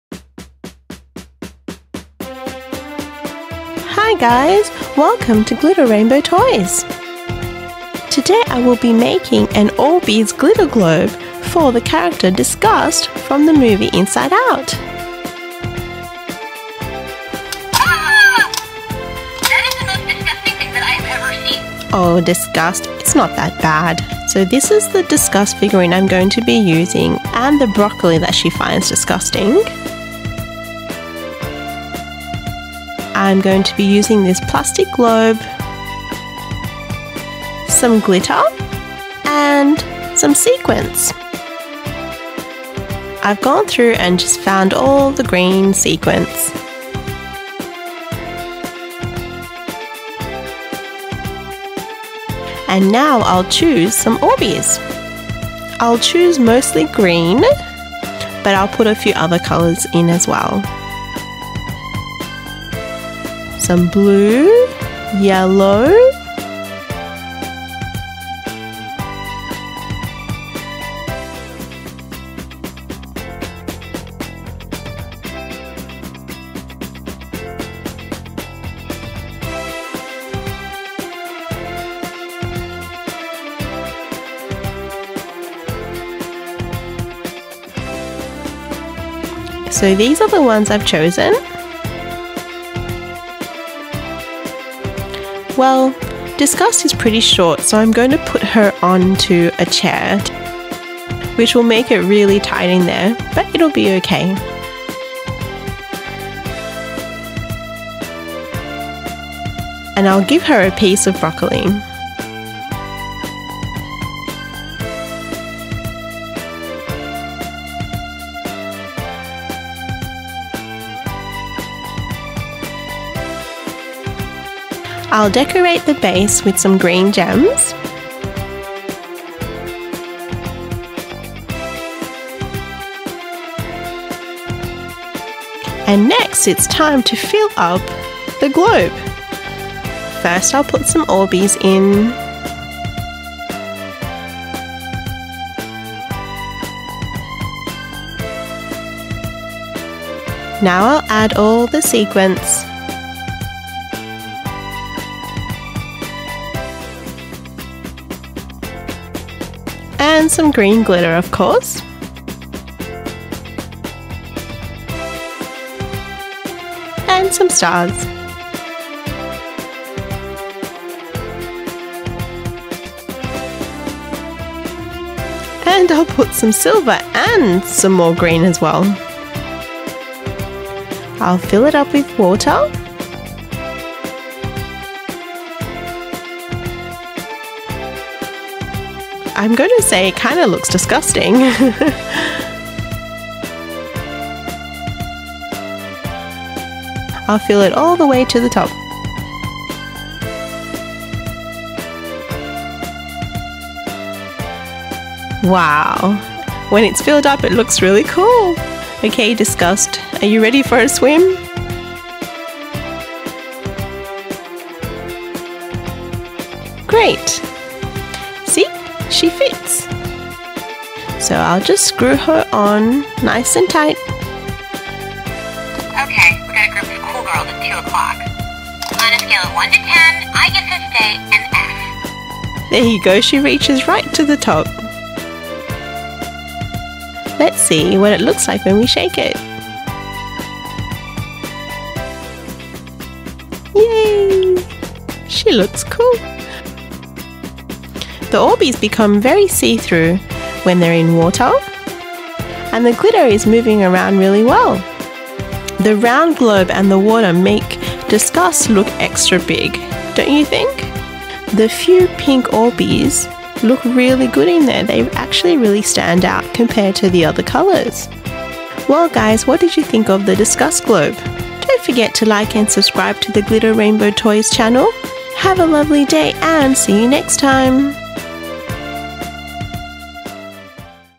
DISGUST GLITTER GLOBE ORBEEZ InsideOut Green Broccoli Gems Toys How-to Make
Disgust Glitter Globe made with Orbeez Toys, Disgust Mini Figure Doll, Glitter, pretend Broccoli, a doll chair and Green Sequins. How to Make an Orbeez Globe using a plastic craft globe and the InsideOut Movie character Disgust. Her own Orbeez Spa.

★ More Inside Out ★

INSIDE OUT DISGUST GLITTER GLOBES Broccoli Dinner Disney Toys How to Make Your Own Green Glitter

Adult Supervision for kids on all craft projects.